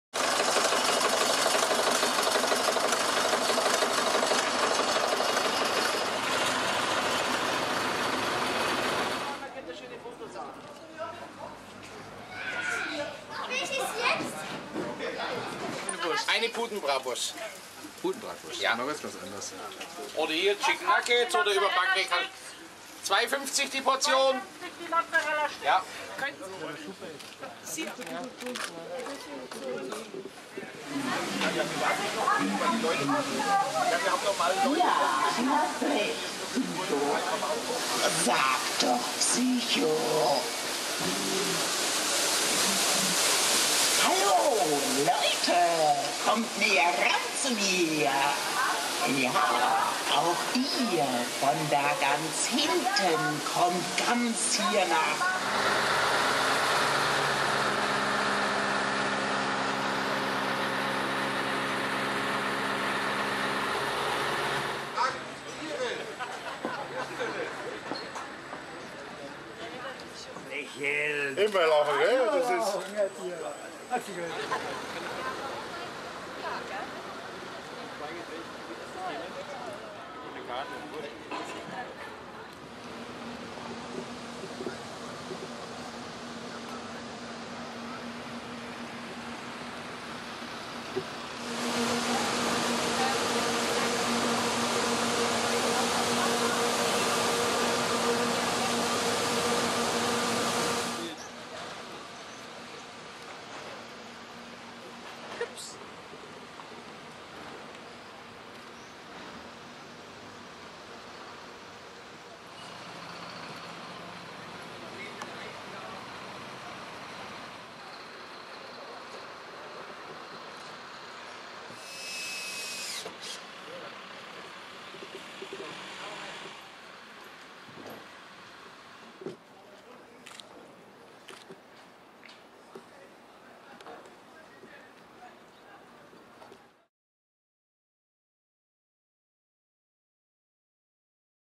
Münchner Frühlingsfest - Impressionen Presserundgang 2 Tage vor dem Auftakt
Münchner Frühlingsfest - Impressionen Presserundgang 2 Tage vor dem Auftakt. Neu sind zum Beispiel Fischer-Vroni's Alt Münchner Steckerlfischbraterei gleich am Haupteingang, die Kinderschleife, erstmalig dabei das 3 Etagen Laufgeschäft Psycho, die Raftingbahn Rio Rapidos, Geflügel Sezialitäten von Joachim Zehle jr, Gebrannte Mandeln von Armin Baumgärtner, und erweitert zeigt sich Zierer's Karussell-Bar mit Gastgarten